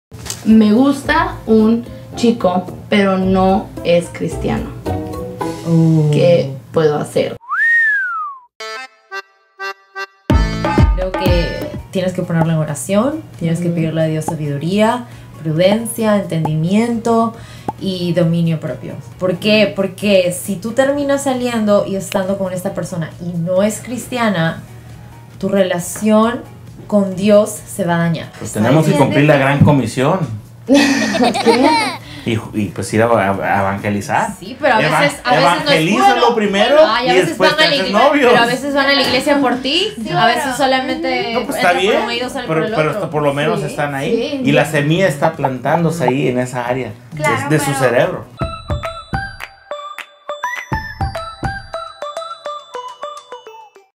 ¿Cómo Puedo manejar una relación con mi pareja que no tiene las mismas creencias espirituales?
"¿Cómo puedo manejar una relación con mi pareja que no tiene las mismas creencias espirituales que yo?"

Puedes encontrar el video completo en YouTube de Mujeres Victoriosas.